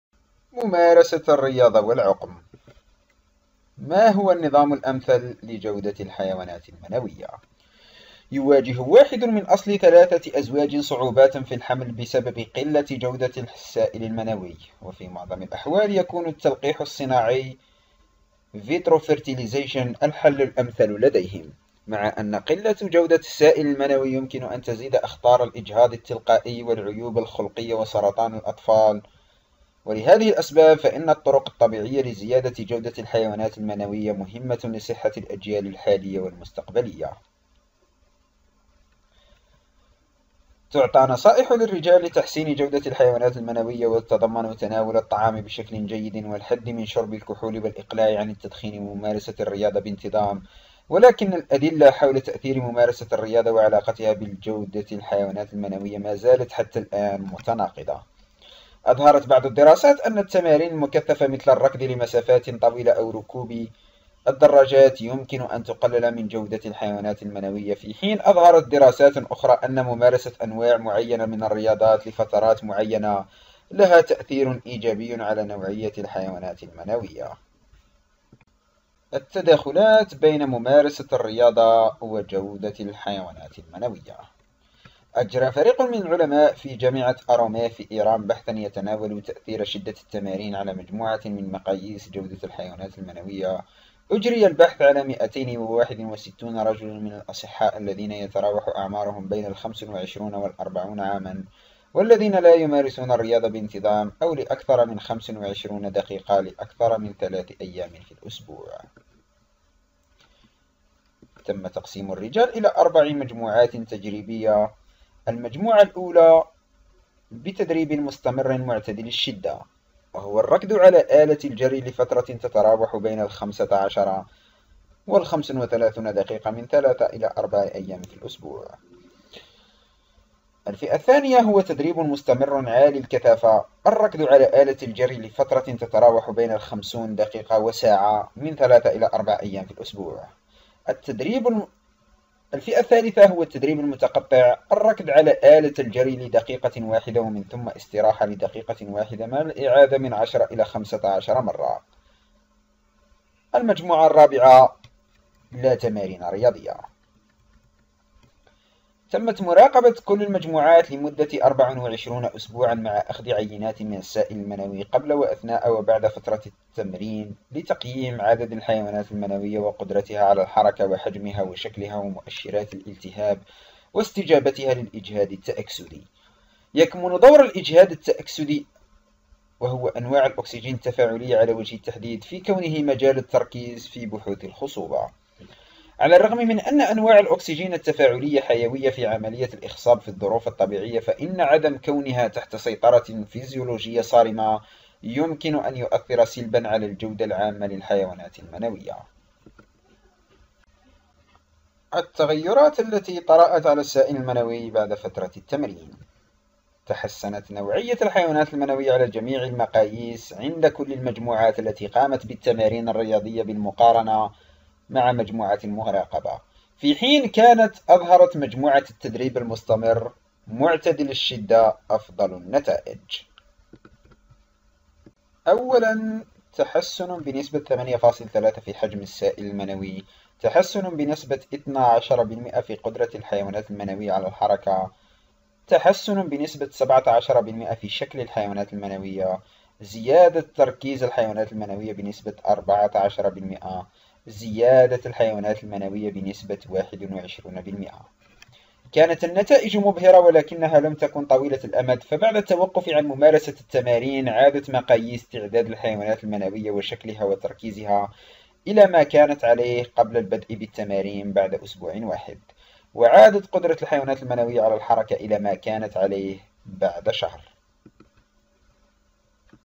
ممارسة الرياضة و العقم : ما هو النظام الامثل لجودة الحيوانات المنوية ؟
يواجه 1 من أصل 3 أزواج صعوبات في الحمل بسبب قلة جودة السائل المنوي، وفي معظم الأحوال يكون (التلقيح الصناعي-in vitro fertilization) الحل الأمثل لديهم، مع أن قلة جودة السائل المنوي يمكن أن تزيد أخطار الإجهاض التلقائي والعيوب الخلقية وسرطان الأطفال، ولهذه الأسباب فإن الطرق الطبيعية لزيادة جودة الحيوانات المنوية مهمة لصحة الأجيال الحالية والمستقبلية.
تعطى نصائح للرجال لتحسين جودة الحيوانات المنوية وتتضمن تناول الطعام بشكل جيد والحد من شرب الكحول والإقلاع عن التدخين وممارسة الرياضة بانتظام، ولكن الأدلة حول تأثير ممارسة الرياضة وعلاقتها بجودة الحيوانات المنوية ما زالت -حتى الآن- متناقضة.
أظهرت بعض الدراسات أن التمارين المكثفة -مثل الركض لمسافات طويلة أو سباقات ركوب الدراجات- يمكن أن تقلل من جودة الحيوانات المنوية، في حين أظهرت دراسات أخرى أن ممارسة أنواع معينة من الرياضات لفترات معينة لها تأثير إيجابي على نوعية الحيوانات المنوية.